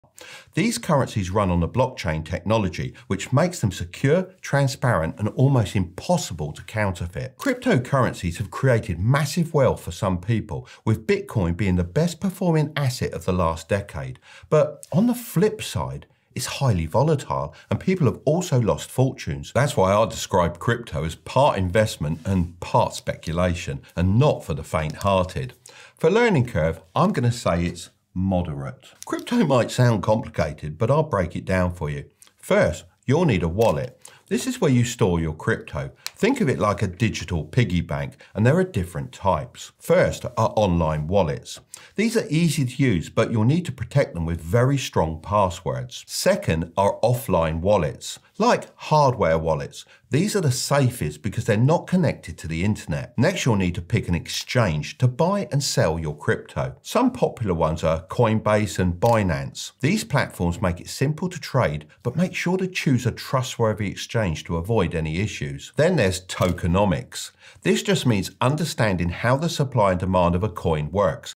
Crypto Explained: Wallets, Exchanges, & Tokenomics Demystified!
Uncover the world of Cryptocurrency! We explain Blockchain Technology, security, and the wealth and volatility of Bitcoin and Ethereum. Understand wallets, exchanges like Coinbase and Binance, and tokenomics. Learn how to navigate the risks and potential gains in our comprehensive guide.